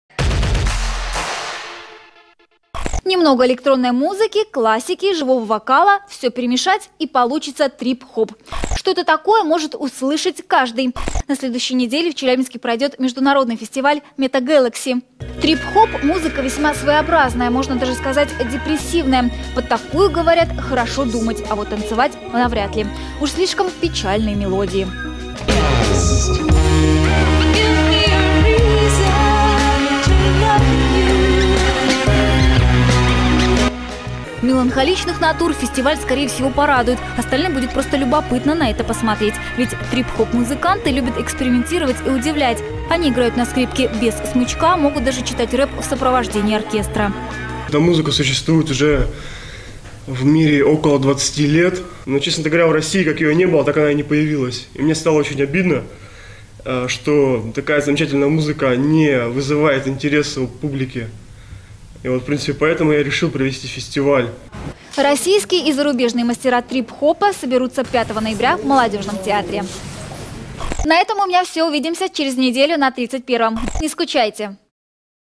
Челябинское телевидение о Metagalaxy Festival 2011 (архив)
Архивный ТВ-репортаж  про единственный в России международный фестиваль трип-хоп музыки  Metagalaxy Festival.
Осень 2011 года.

(проект лейбла Cuntroll)